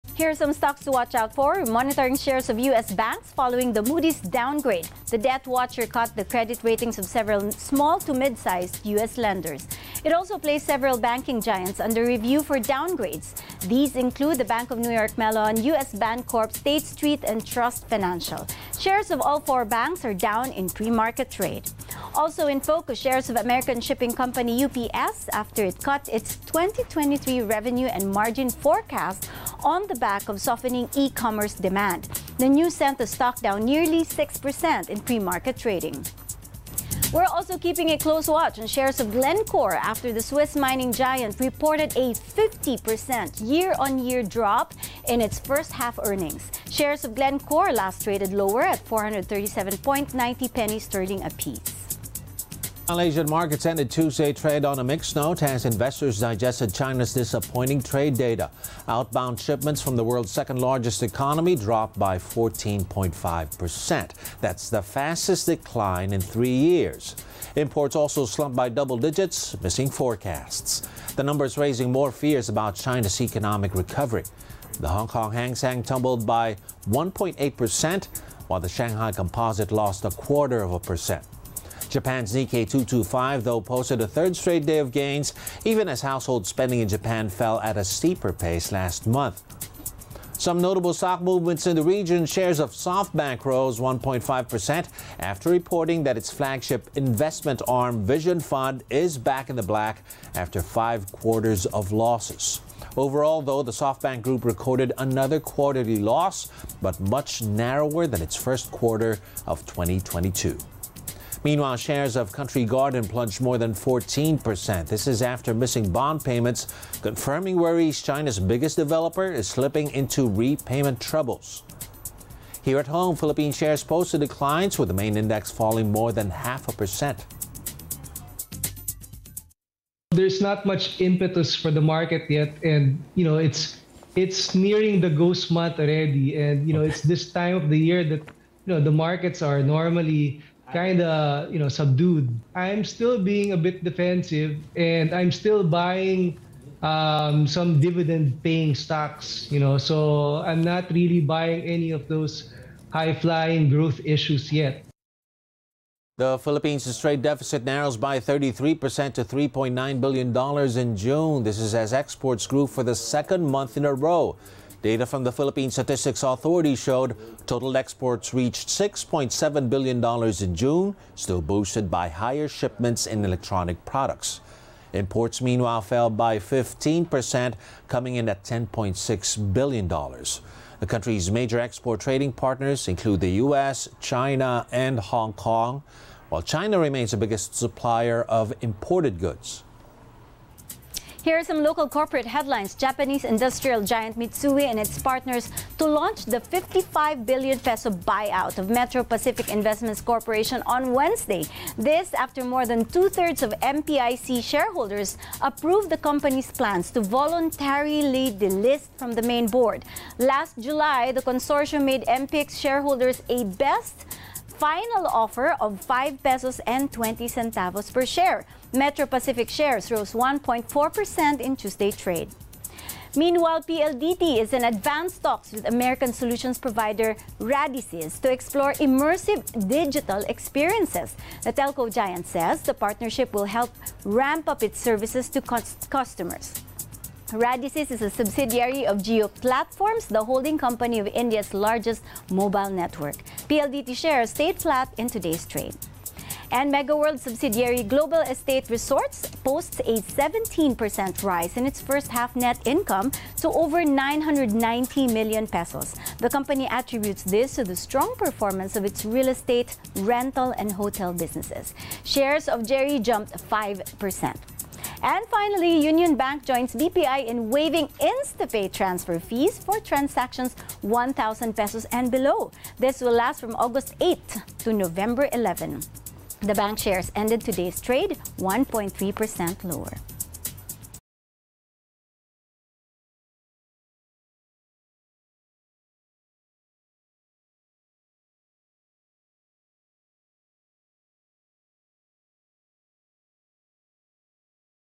Glencore down after profits halve | ANC
Business Outlook: Here are some stocks to watch out for.